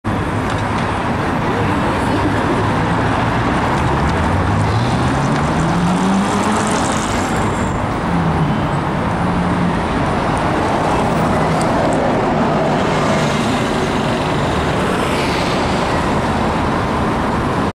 Троллейбус БКМ 321 борт 2409 по маршруту 22
Дата съёмки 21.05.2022 г.Санкт-Петербург, Невский проспект